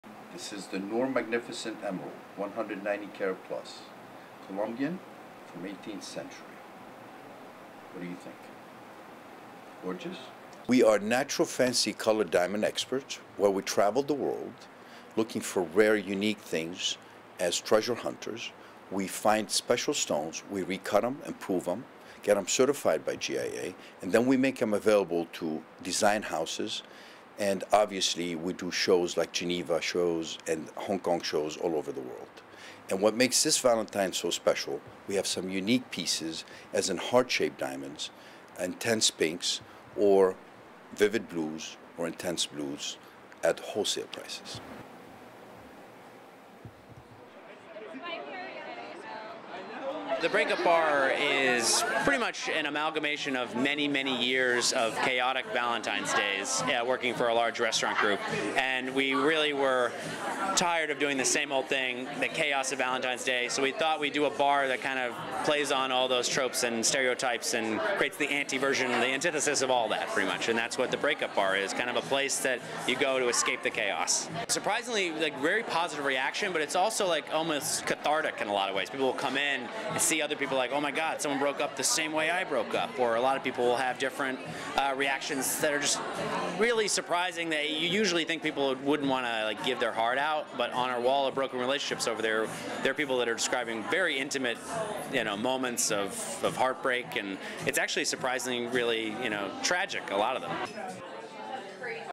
Diamonds or disappointment for Valentines Day?
(13 Feb 2019) It's your choice for Valentine's Day – expensive diamonds or bitter disappointment. If million-dollar jewelry from Beverly Hills isn't in the cards, you can drown your romantic sorrows at the BreakUp Bar in Los Angeles.
Eli and Co. Fancy Diamonds Inc. in the heart of Beverly Hills, is one of the world's rarest high-end diamond and gemstone wholesalers for the Valentine who already has everything.
This year's Valentine's gifts include a 10 carat Natural Fancy Pink Diamond ring for 29 million dollars.
Or if the budget doesn't stretch quite that far, how about the Natural Fancy Purplish Pink Diamond ring for 400 thousand dollars.
The team scours the globe looking for the world's most expensive and most rare diamonds and gemstones.
Company CEO Eli Noor helps one woman try on the 18th century historical Noor Magnificent Emerald.
At over 190 carats, it will set you back a massive 55 million dollars US dollars.
Meanwhile, BreakUp  is a popup bar which let's people melt in their melancholy and bathe in their tears, but it's also about catering for people who want to have fun and forget the heartbreak.
The bistro-style restaurant has been sold out since the day it opened with people queuing up to get in each night.
The BreakUp Bar's owner Evan Charest says its been extremely popular.
"BreakUp Bar is pretty much an amalgamation of many, many years of chaotic Valentine's Days. Working for a large restaurant group we really were tired of doing the same old thing, the chaos of Valentine's Day so we thought we'd do a bar that kind of plays on all those stereotypes and creates the anti-version and antithesis of all that pretty much, and that is what the BreakUp Bar is, kind of a place to go to escape the chaos" says Charest.
The BreakUp Bar features a "Wall of Broken Relationships", covered with notes, texts and conversations from an ex-boyfriend or ex-girlfriend.
Unsurprisingly most are angry, a good way to vent your frustration and sadness.
Everyone is encouraged to write a note for the wall before they leave.